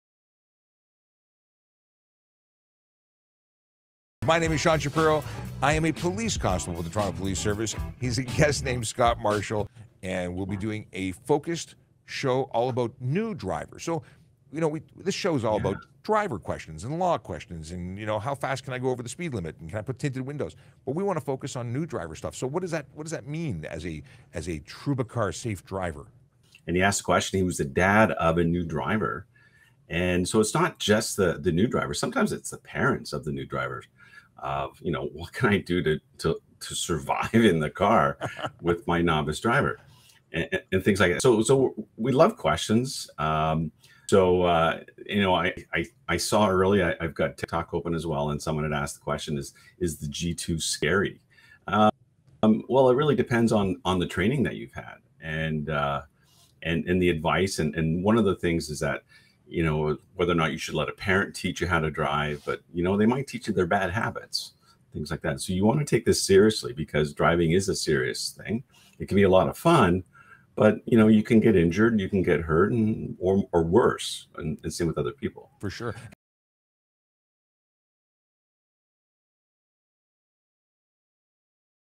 Trubicars in conversation with @voiceovercop
🚗 Buckle up for a thrilling ride of knowledge with Sean Sharipo, a seasoned Constable from Toronto Police, as he converses with the driving expert, Scott Marshal from Trubicars! 🌟 Get exclusive insights on road regulations, safe driving strategies, and some real-life experiences from the pros. This is the perfect chance to sharpen your driving skills and view the road through the eyes of an expert. 🎯 Not just informative, it's sure to get your adrenaline pumping with behind-the-wheel excitement. 🚀 So, gear up, hit the gas, and immerse yourself in the world of professional driving! SafetyFirst DrivingMastery RoadRulesRevealed🚦🔥